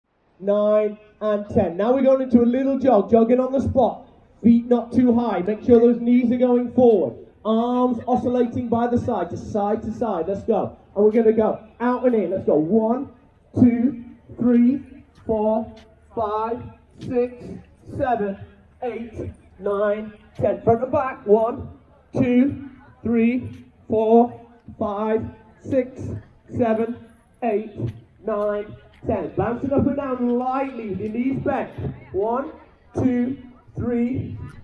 Electrolux Home Run 2019
The Warm-Up.. 🤸‍♀️🤸‍♂️
Event ; Electrolux Home Run 2019
Date: 14 July 2019 (7am Flag Off)
Venue: Setia Alam City
Distance: 10km, 5km, 2.8km, 2km & 1km